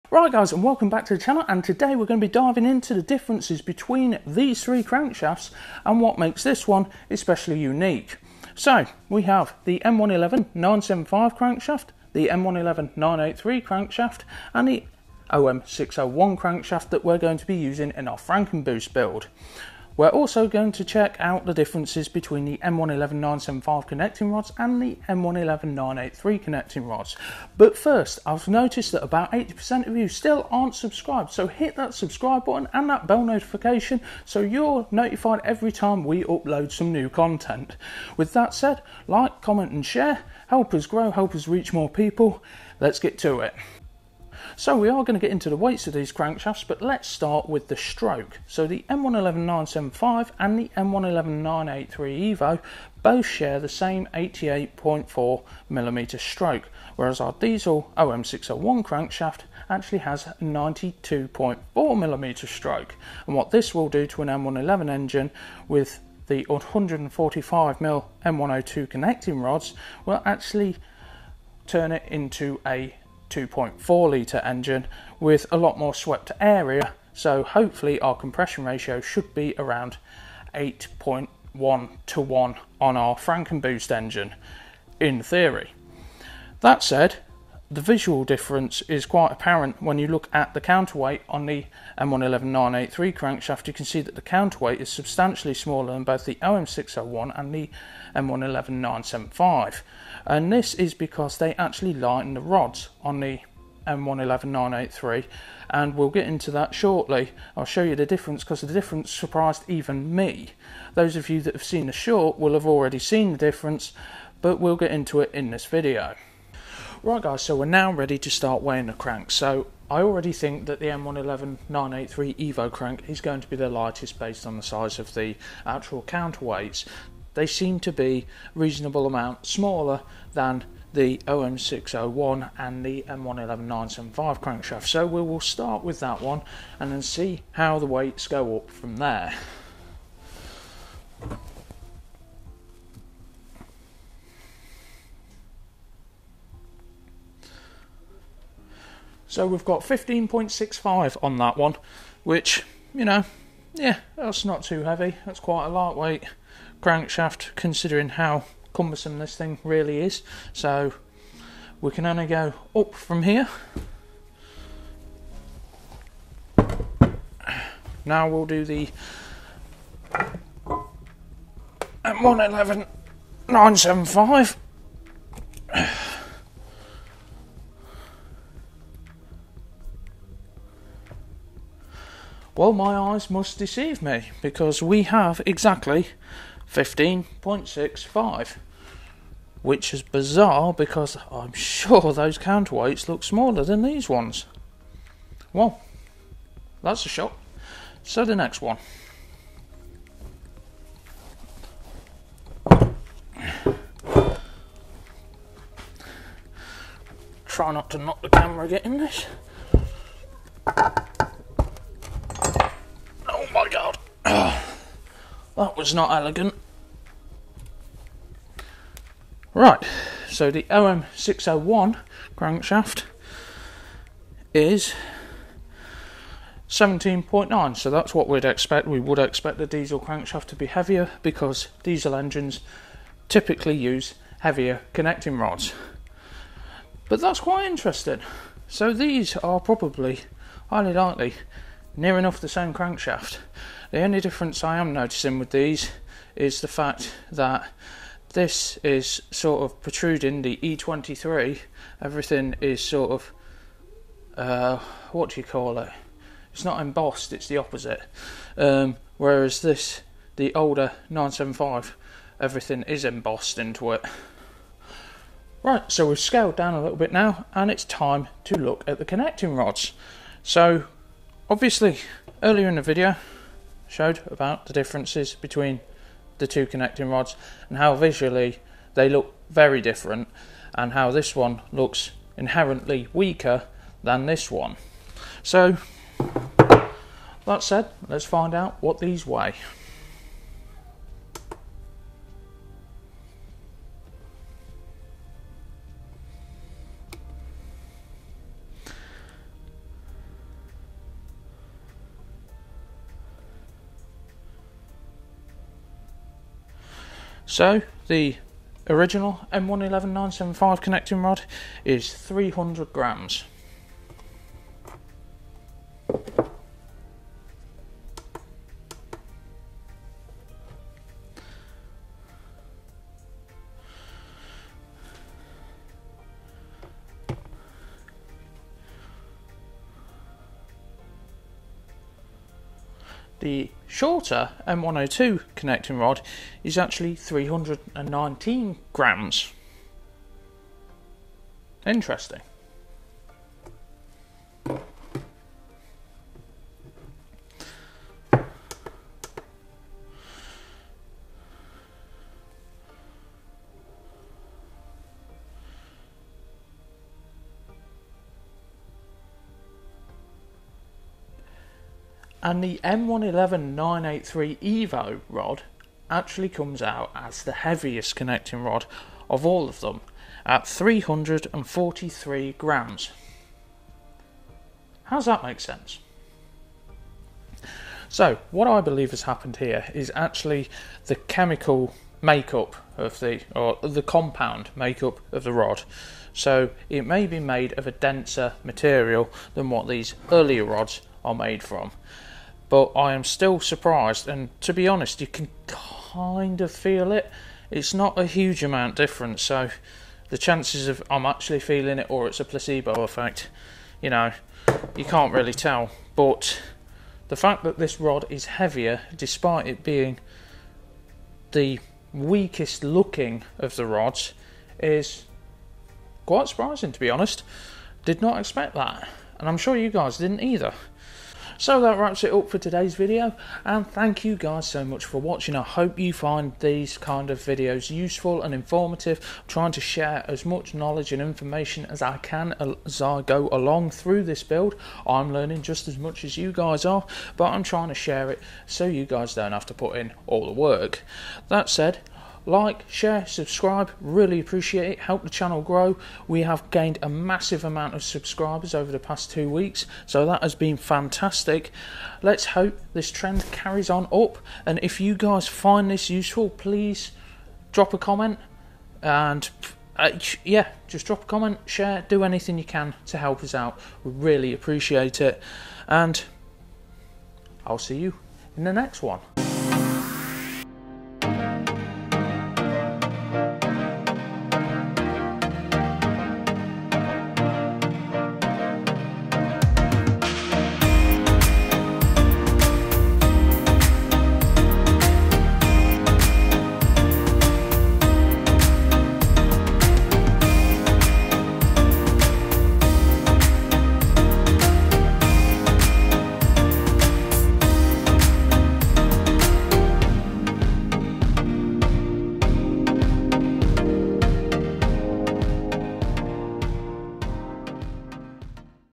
M111 vs M111 EVO Crankshaft & Rods – What Actually Changed?
In this video, we take a deep dive into the internal differences between the Mercedes M111 and M111 EVO engines — specifically the crankshafts and connecting rods. Whether you're building for boost or just curious how Mercedes upgraded these powerplants, this is a must-watch comparison.

These engines were used in a wide range of Mercedes-Benz models from the 1990s to 2003, including:

E-Class, CLK and SLK models

Ml230 and Sprinter vans aswell as the Vito and V230

Kompressor variants (supercharged M111s)

We’ll show you what changed in the EVO variant, why it matters for strength and reliability, and how it impacts turbo builds like our custom M111 Frankenboost setups.

If you're planning a build, tuning your M111, or just love learning how these legendary 4-cylinder Mercedes engines evolved, this video is for you.